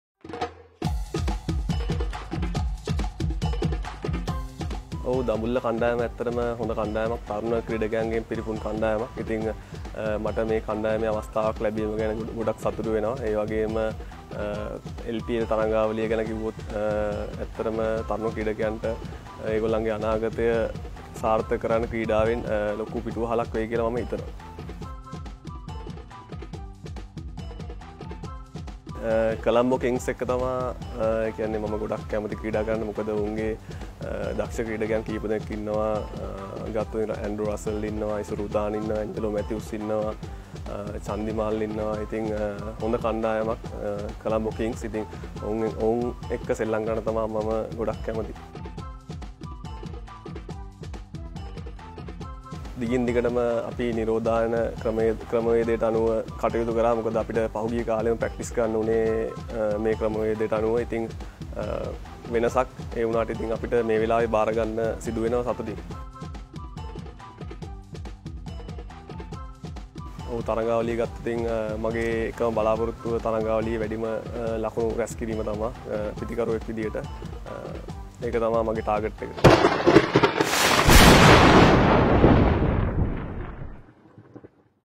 Dambulla Viking Caption Dasun Shanaka LPL 2020 | Lanka Premier League 2020 | Everyday Cricket
This Video About Dambulla Viking Captain Dasun Shanaka's Comment about LPL 2020